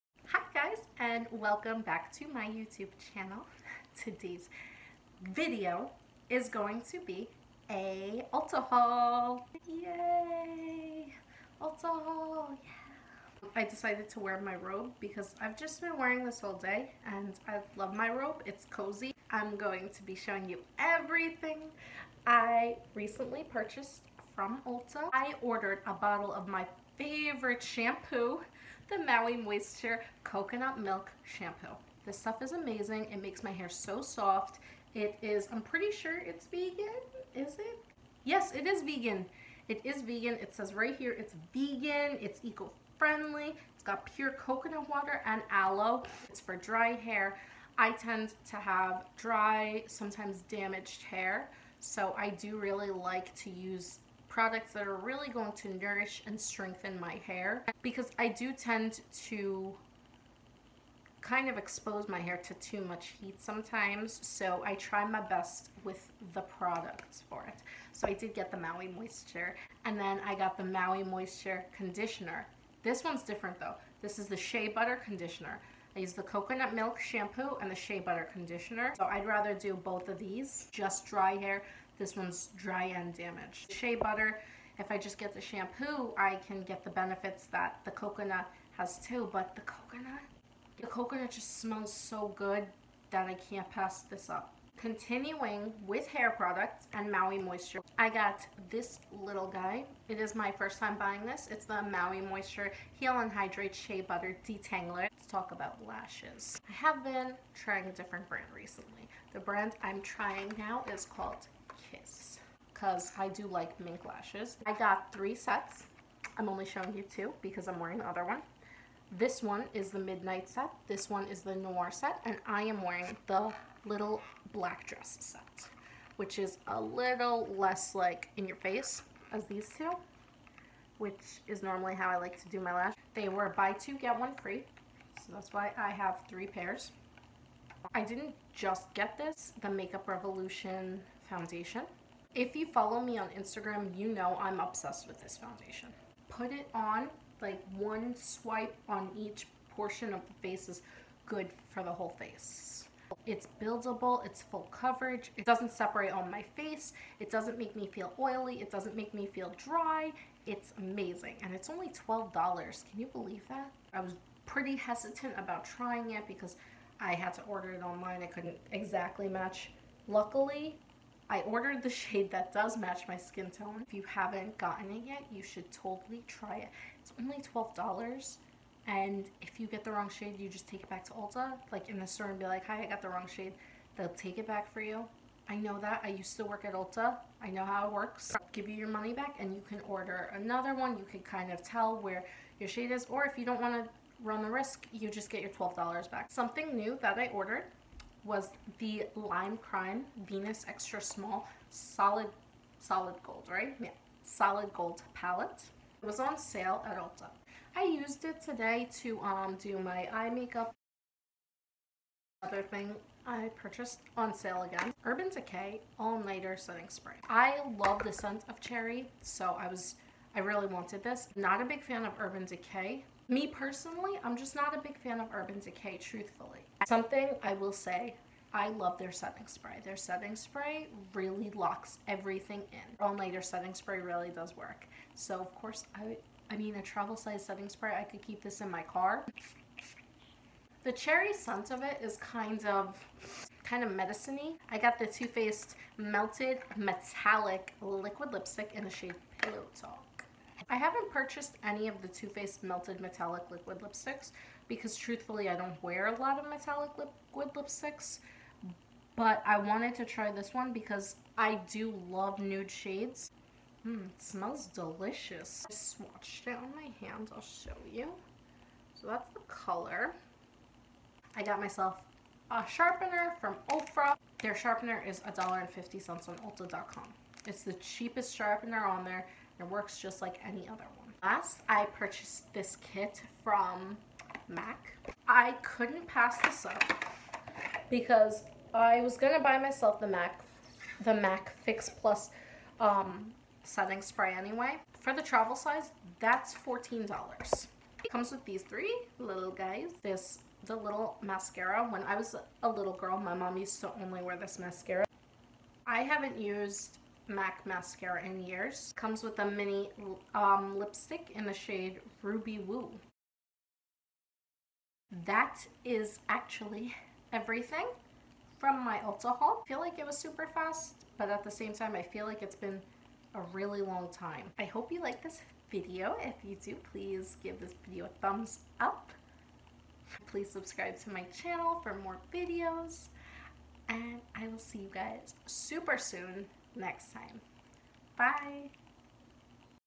First 2019 Ulta Haul | MaiTai Makeup
If you are new to my channel I invite you to join in being one of my friends. My arms are open to all YouTube beauties!
 For Christmas I got $75 worth of Ulta gift cards and I’ve just gone 3 months without buying any makeup so I decided it was time for an Ulta Haul! For my own enjoyment and yours! 👏🏻👏🏻  and over $30 of Ulta points redeemed! 🙌🏻  Thank God for Ulta gift cards or I wouldn’t have gotten these beauties

Products Purchased
Maui Moisture Coconut milk shampoo $8.99
Maui Moisture Shea butter conditioner $8.99
Maui Moisture Shea butter detangler  $8.99
Kiss Lash Couture “Little Black Dress” $5.99
Kiss Lash Couture “Noir” $5.99
Kiss Lash Couture “Midnight” $5.99
Lime Crime Venus XS solid gold palette $14
Urban Decay All Nighter setting spray (🍒 travel size) $10.50
OFRA cosmetic pencil sharpener $1.50
MAC “shiny pretty things all star kit” $15